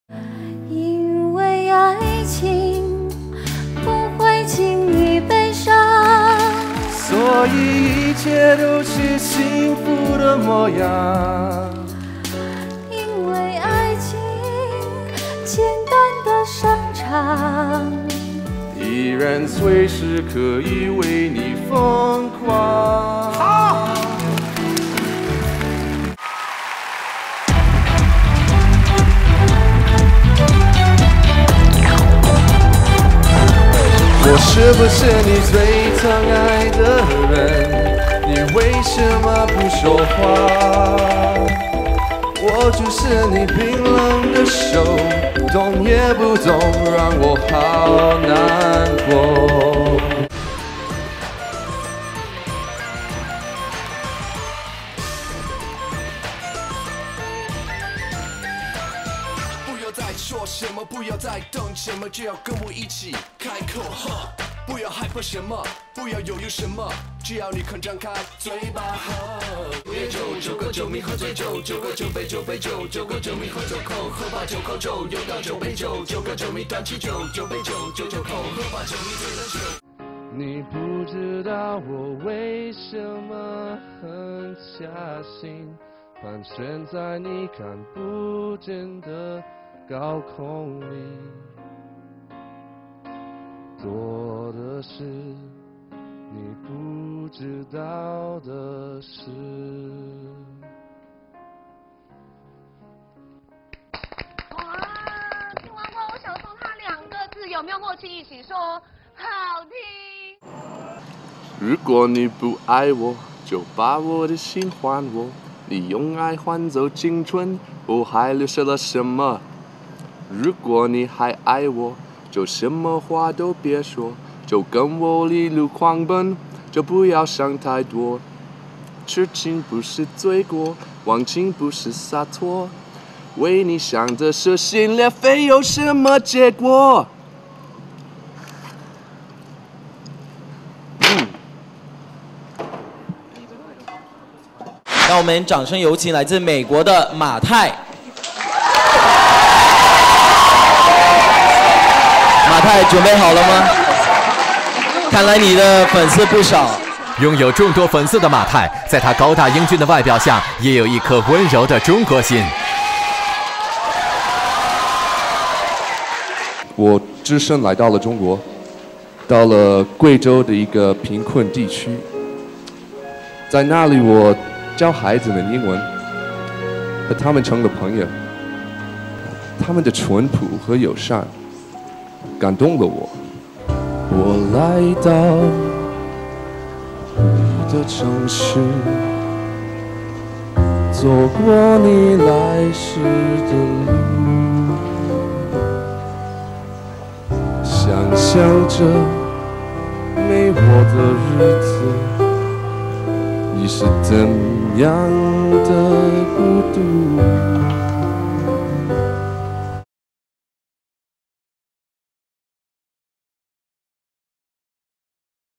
Matt William Knowles (MaTai) Sings in CHINESE
Various Clips of Matt William Knowles singing on Chinese Television